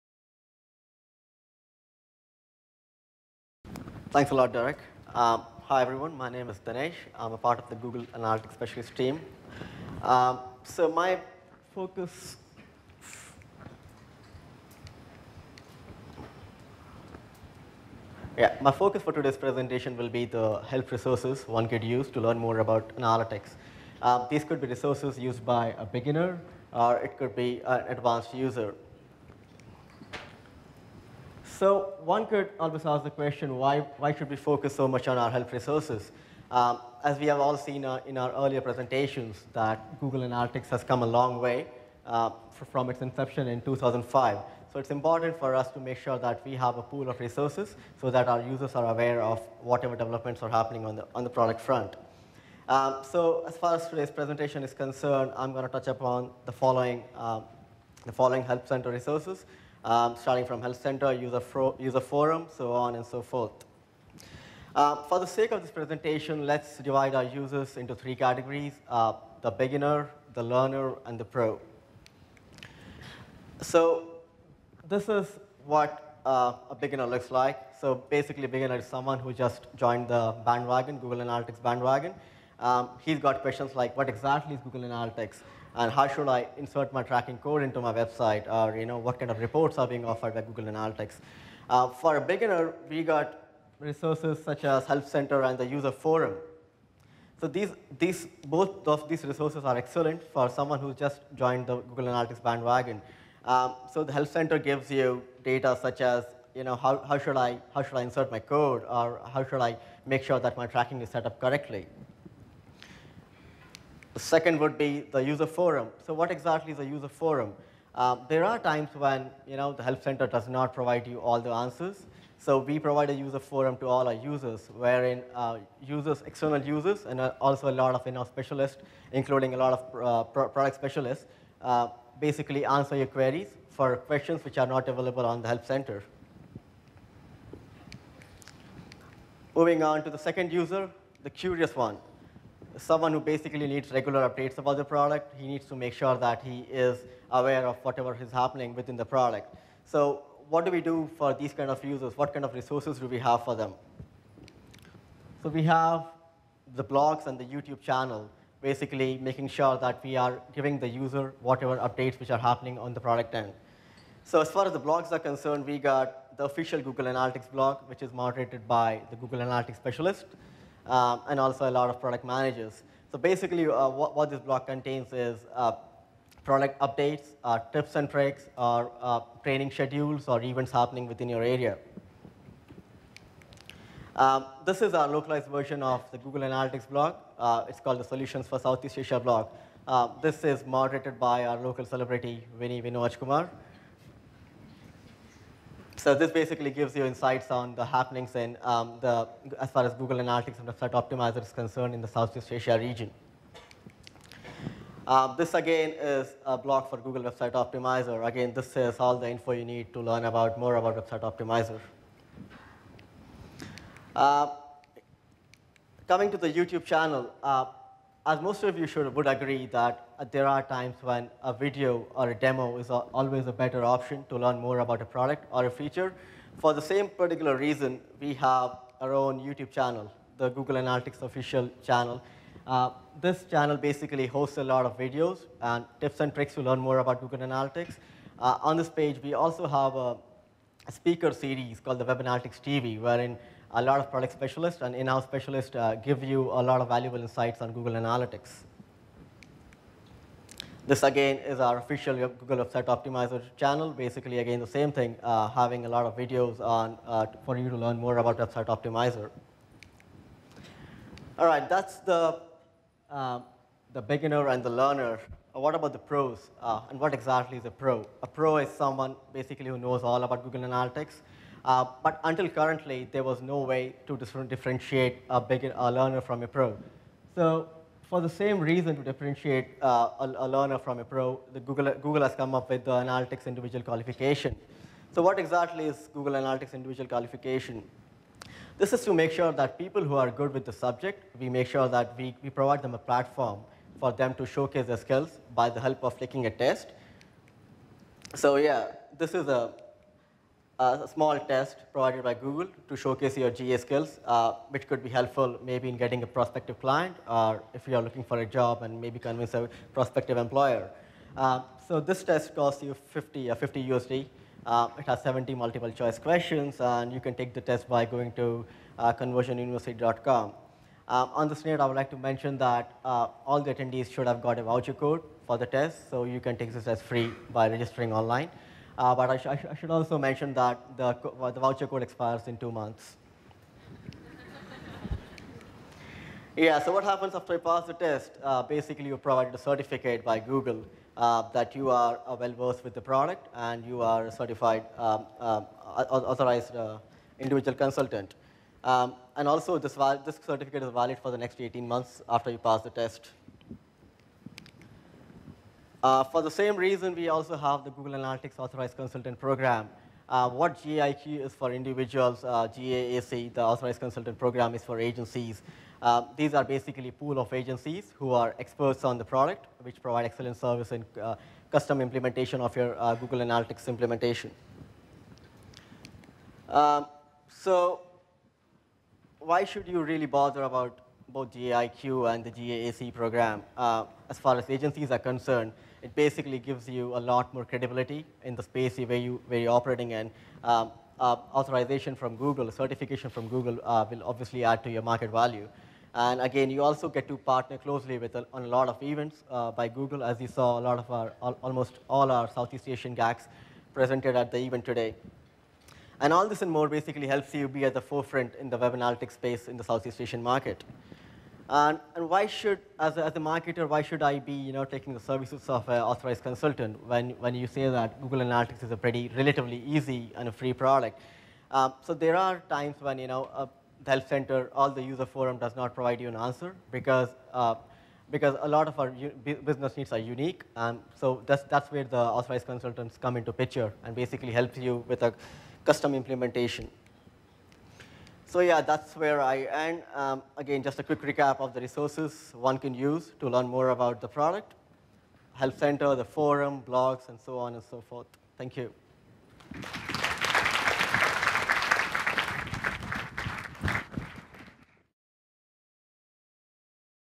Google Analytics Master Class Singapore: Google Analytics Resources (Dinesh Jain, Google)
Dinesh Jain, Google Analytics Specialist, Google India

Inspired to start making full use of Google Analytics? Learn more about the available educational resources

Find out more about what's happening with Google Analytics in Southeast Asia